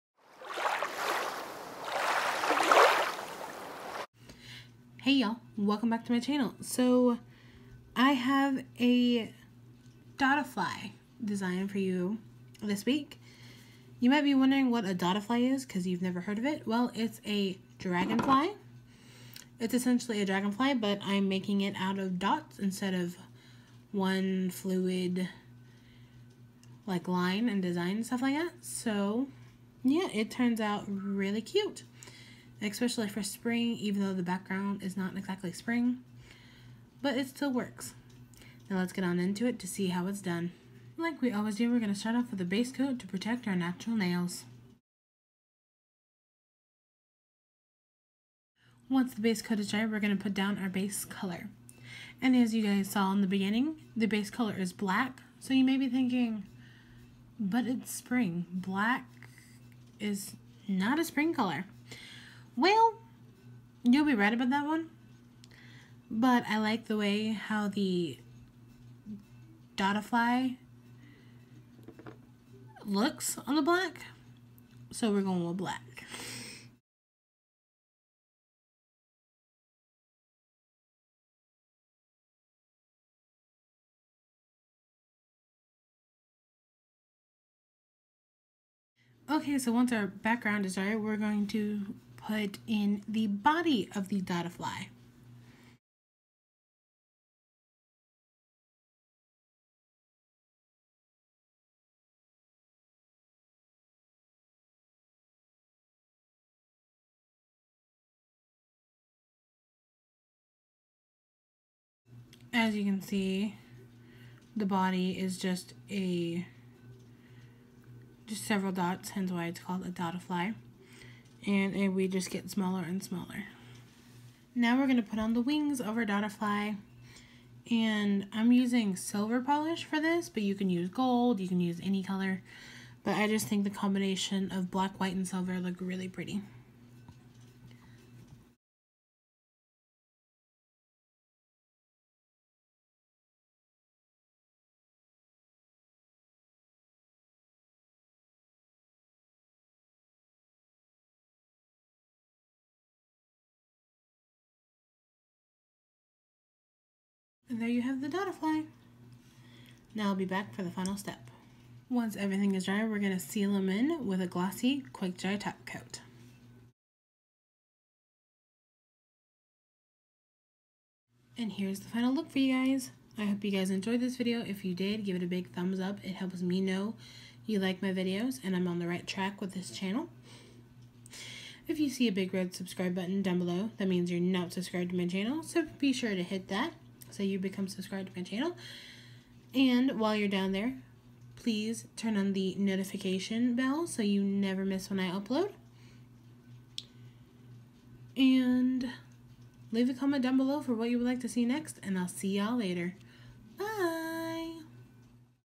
dottifly dragonfly
a cute little dottifly dragonfly came to visit and stayed a while! this design was super easy to do and i love how the colors go together.

silver polish